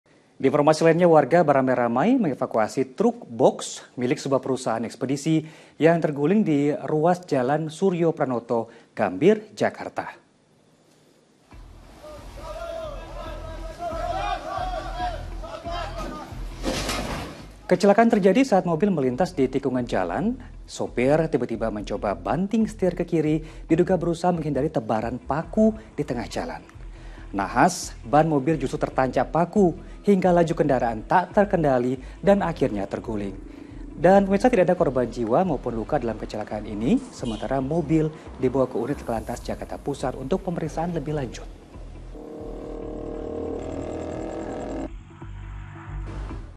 Mobil Boks Terguling di Jalan Suryopranoto saat Menghindari Tebaran Paku | Patroli
Sebuah mobil boks terguling di ruas jalan Suryopranoto, Gambir, Jakarta Pusat. Diduga pengemudi kehilangan kendali saat berusaha menghindari tebaran paku.